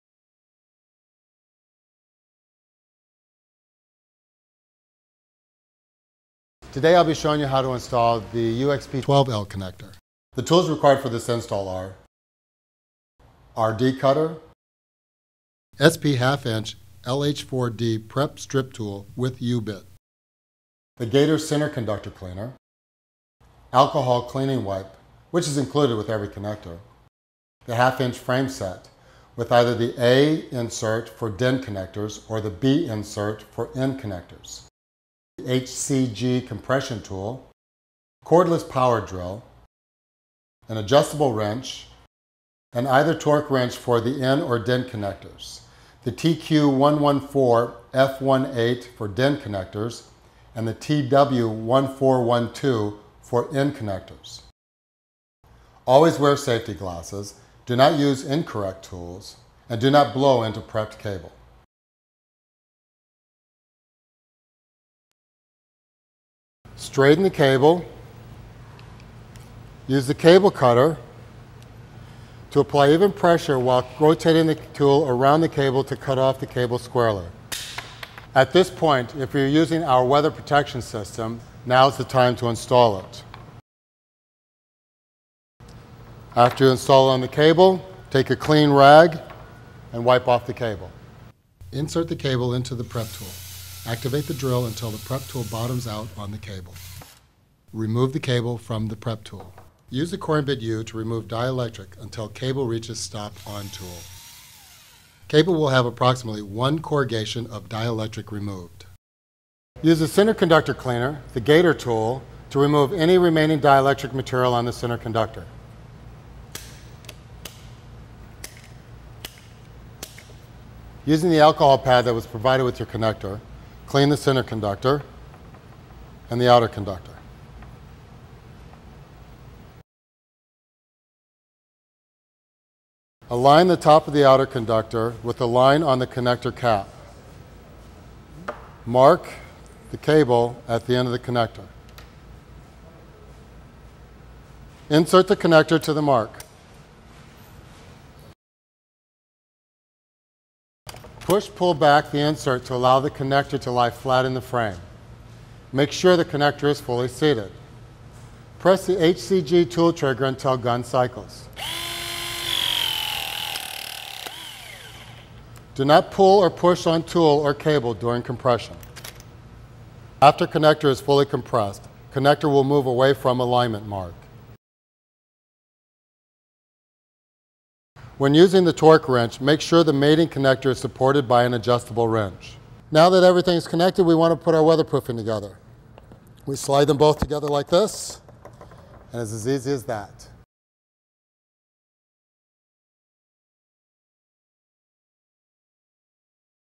JMA 1/2" Annular LS Connector Installation
How to install JMA's UXP-XX-12L compression connectors.

Cable Supported:
LS LHF 12D